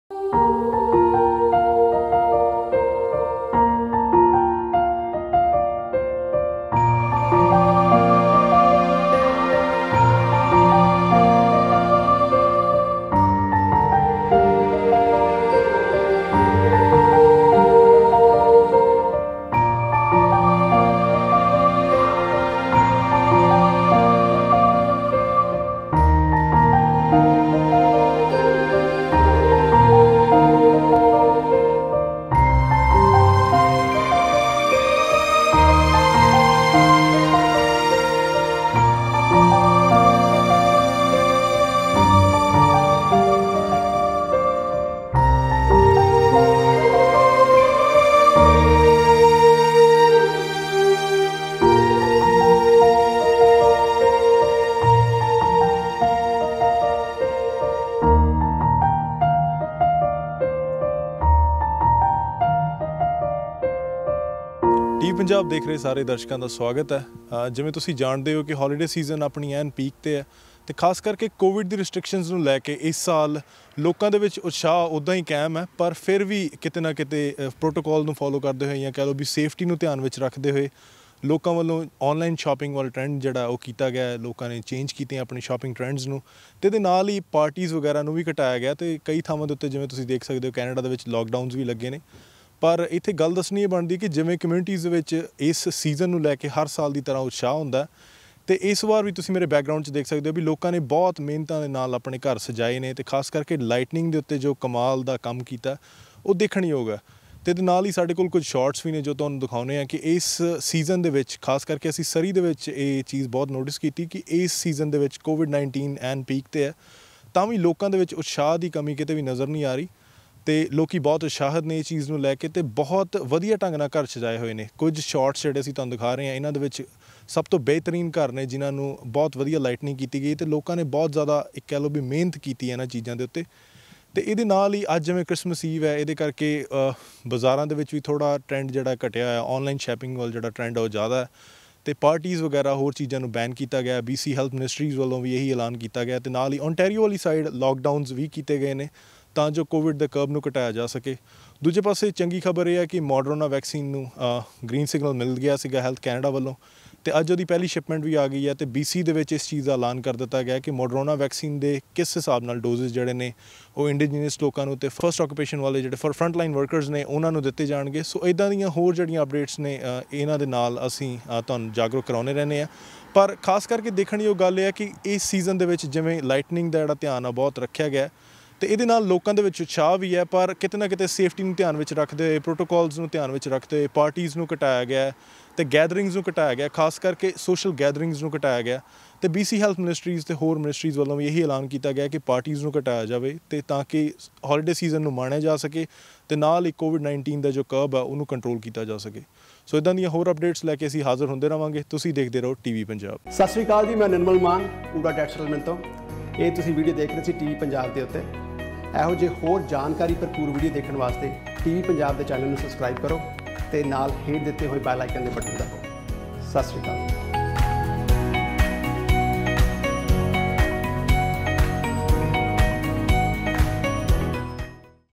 Merry Christmas Greeting From Vancouver and Surrey
During COVID restrictions people spent more money and time to decorate their homes. TV Punjab in this report showing you few of the well decorated homes.